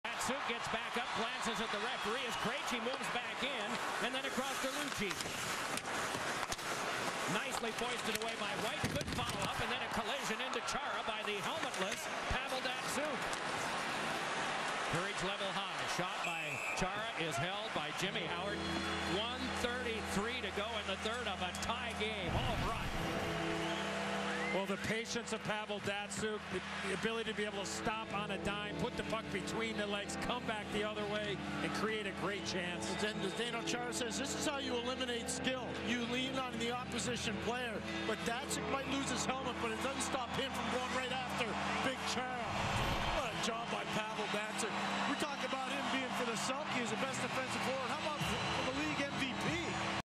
Pavel Datsyuk playing tough vs Zdeno Chara [HD]
Datsyuk had a MVP-caliber performance in a game between the Detroit Red Wings and Boston Bruins on Friday, November 25th, 2011 at the TD Garden in Boston, Massachusetts in the Thanksgiving Showdown.